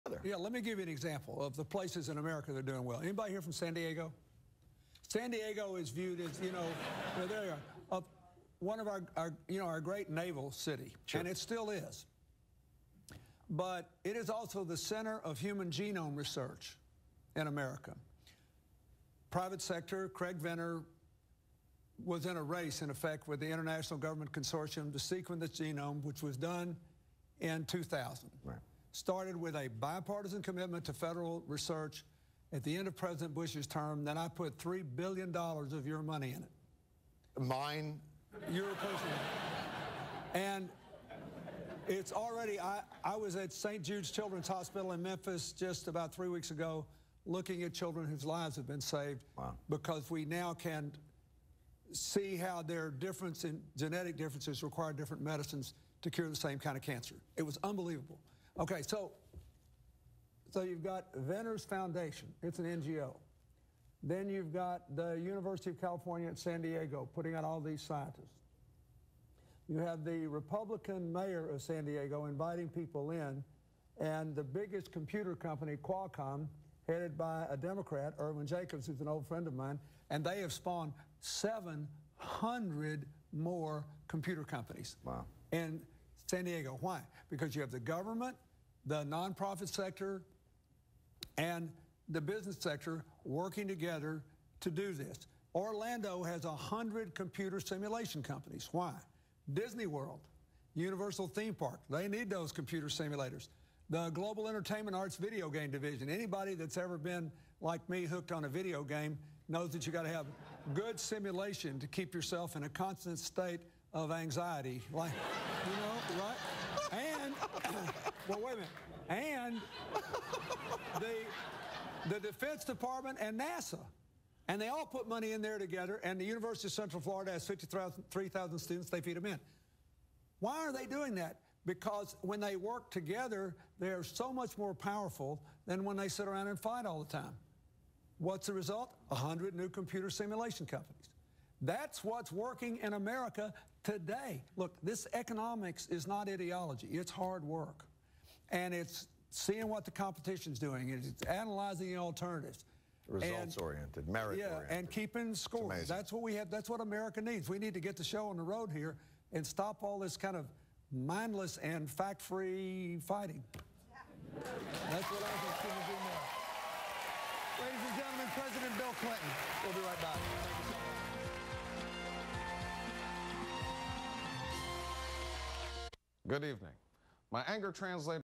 President Clinton Gives Props to San Diego
Former President Bill Clinton praises efforts in San Diego for businesses, politicians and education facilities to work together, across party lines.